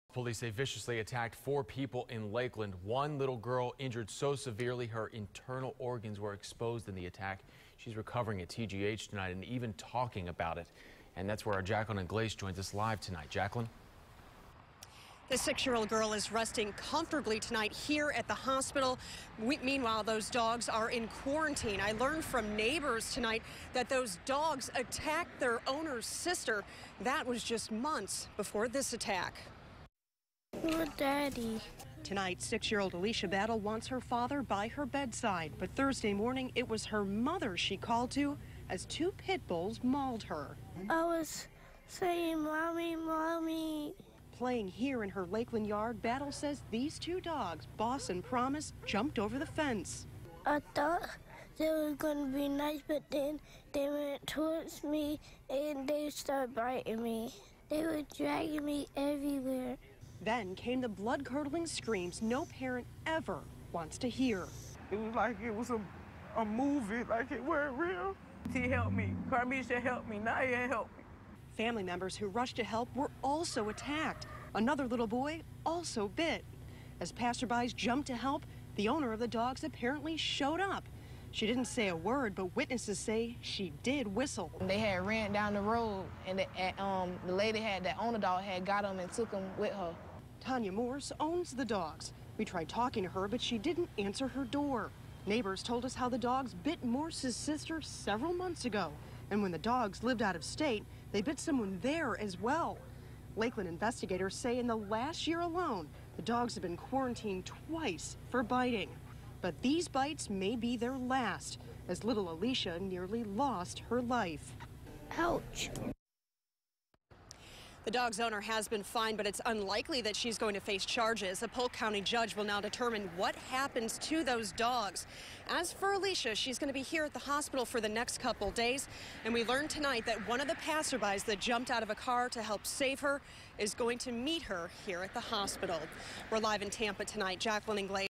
Lakeland dog attack
Two dogs attack four people in Lakeland, including a six-year-old girl who suffered severe injuries.